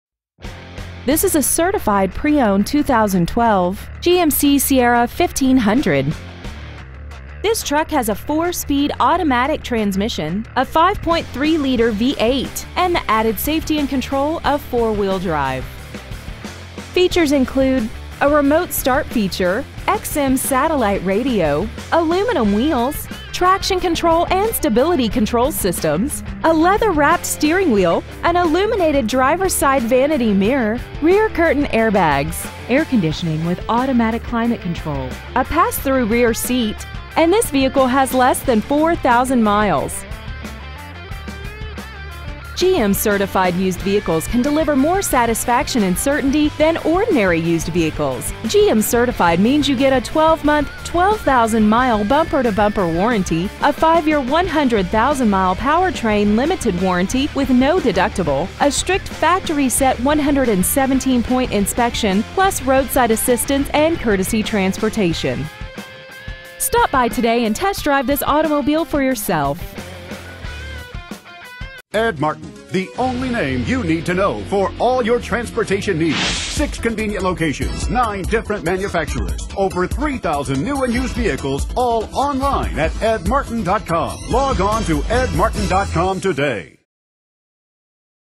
Preowned 2012 GMC Sierra 1500 Carmel IN 46032
We have been honored to serve the Carmel IN area , we promise that your experience at our dealership will exceed your expectations ! Year : 2012 Make : GMC Model : Sierra 1500 Engine : Gas/Ethanol V8 5.3L/323 Trans . : Automatic Exterior : Fire Red Miles : 3,347 Interior : Ebony Stock : 5P3701 Ed Martin Buick Pontiac GMC North Michigan Road Carmel , IN 46032